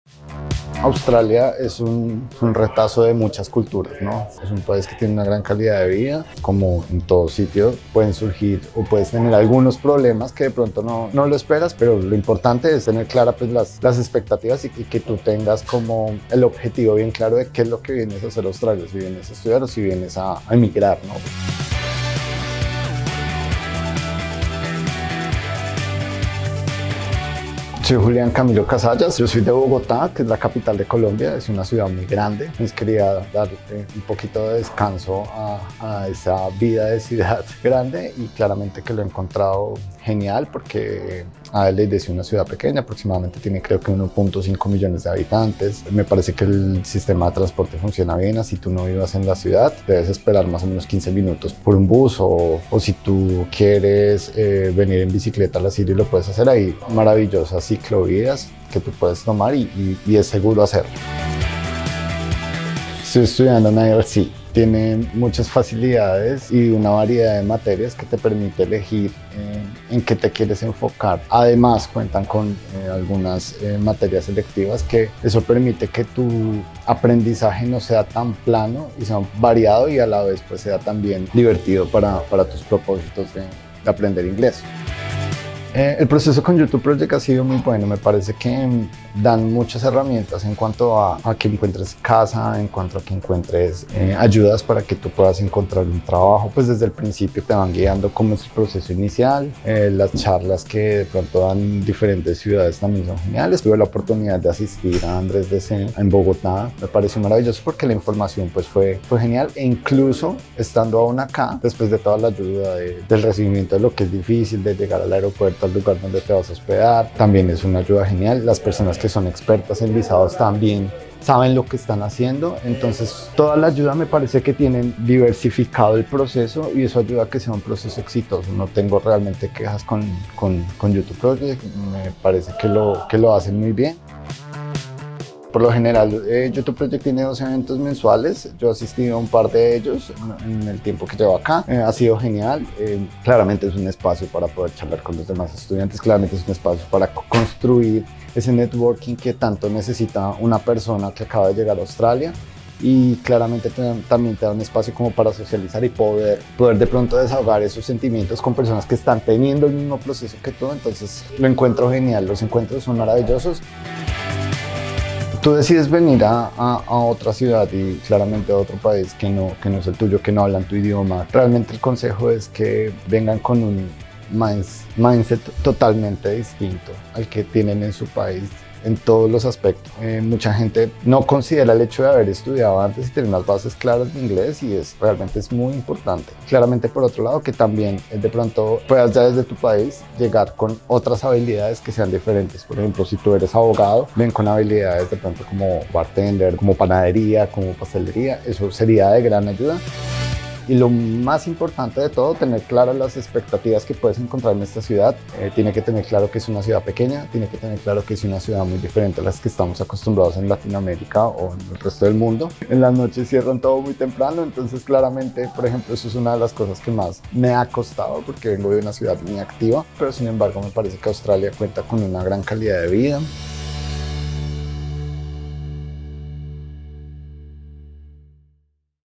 ¿Cómo es la vida de un estudiante en Adelaida?
Julián Camilo nos cuenta cómo es la vida en Adelaide, la capital de South Australia y todos los consejos para estudiar y vivir en esta ciudad australiana.

¿Te gustaría estudiar en Adelaida? ¿Qué ciudad australiana crees que tiene más oportunidades?

Si quieres estudiar tu curso de inglés en Adelaida, ¡escríbenos y te ayudamos con todo el proceso!